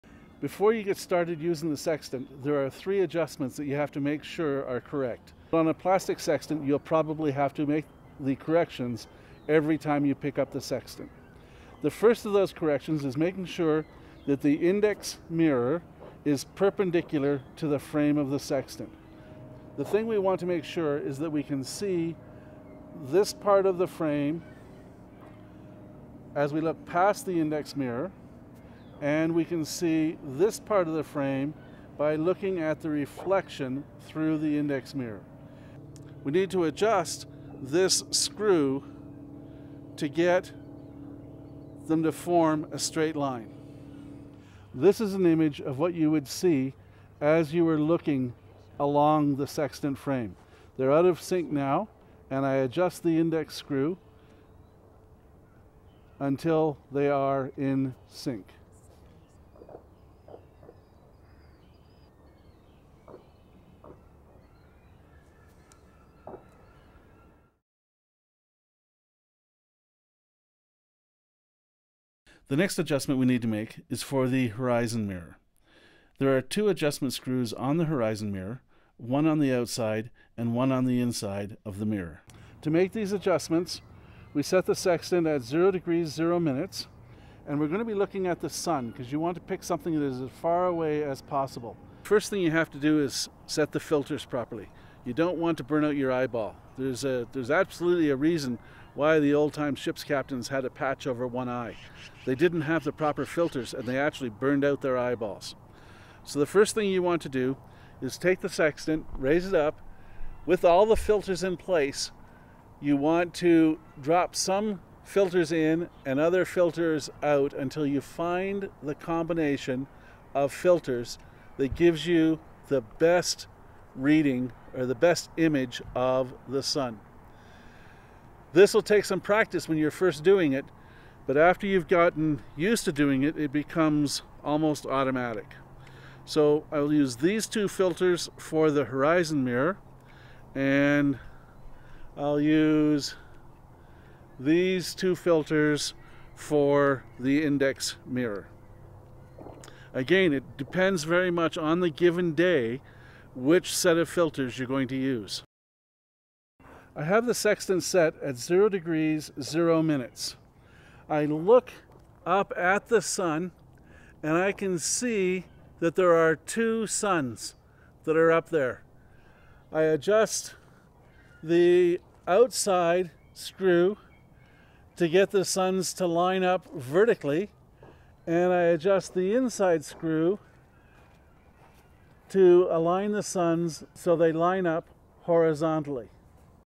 Adjusting a Davies 15 Sextant for Errors
Manually adjusting a Davies 15 Sextant to remove errors. Video exert content courtesy of James Cook from his YouTube video "Calibrating and Using A Sextant"